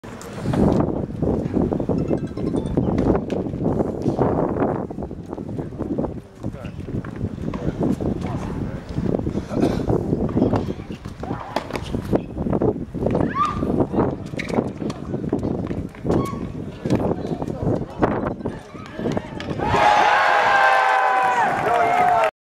Fed Cup: Israel vs Luxembourg - Julia Glushko match point against Anne Kremer
This is the match point of Julia Glushko (Israel) against Anne Kremer (Luxembourg) during the 1st day of Group I 2011 Europe/ Africa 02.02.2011.